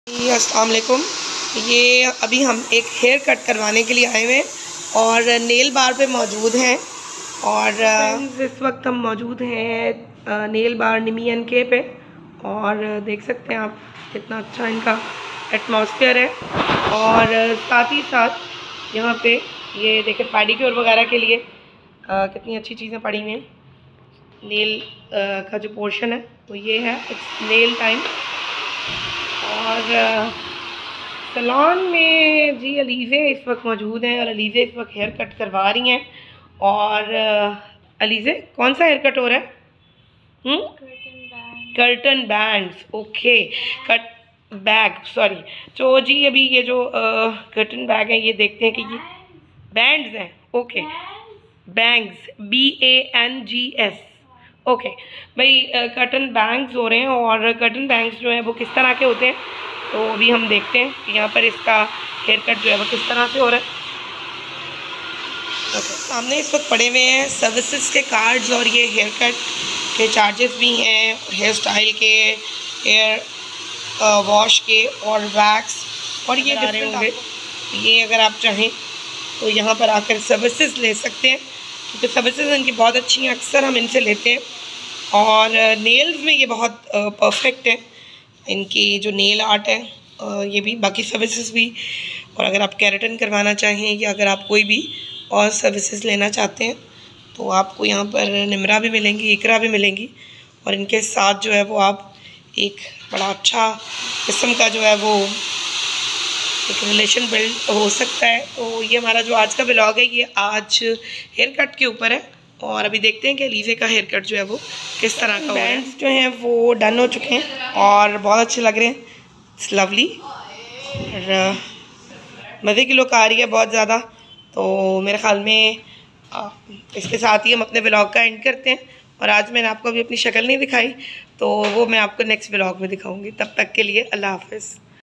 Haircut by Nimi & k
Amina Usman's daughter Alizeh planed  a haircut and she came to  Nail Bar by Nami & K. There Iqra got the best curtain bangs hair style from a hair stylist. Nami & K has the best nail bar services and the charges for services are very reasonable.  This Nailbar &  Salon is located near Roop Singhar PIA Road lhr